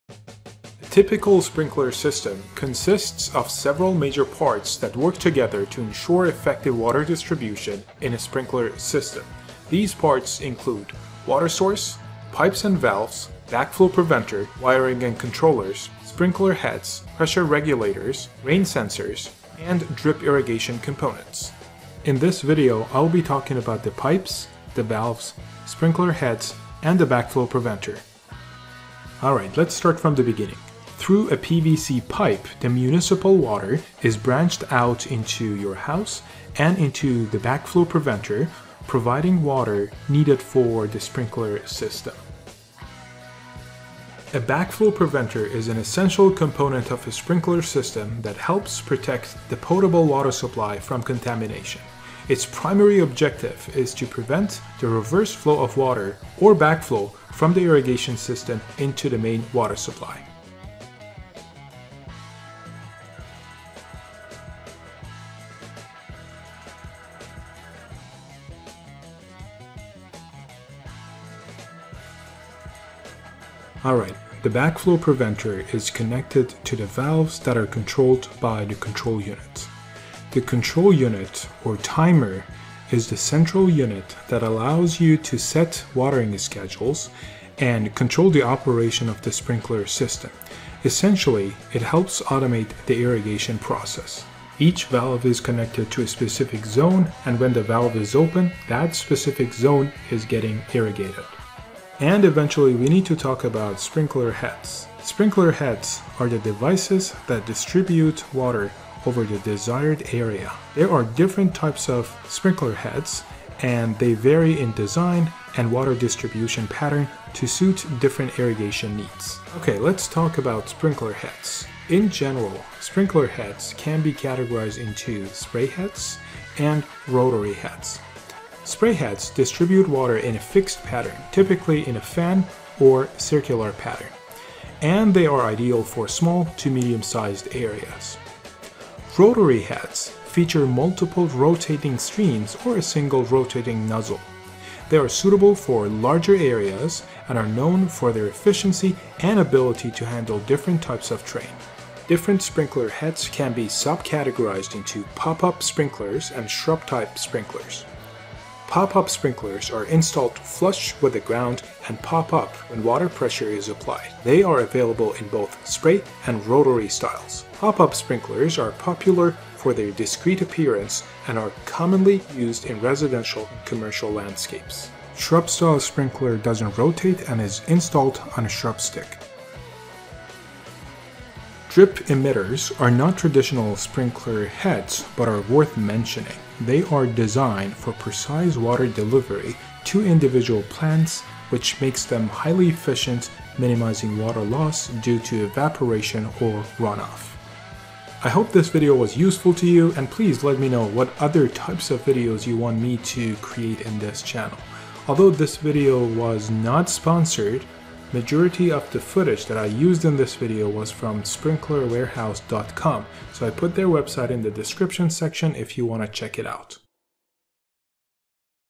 Sprinkler System Essentials: Understanding the Basics of sprinkler irrigation Systems
In this informative video, we delve into the fundamentals of sprinkler systems. From the key components to their functioning, join us as we explore the essential elements that make up a successful irrigation system. Gain valuable insights into valves, sprinkler heads, controllers, and more, empowering you to optimize your watering efficiency and maintain a healthy, vibrant landscape. Whether you're a beginner or an experienced gardener, this video is a must-watch for anyone looking to master the fundamentals of sprinkler systems.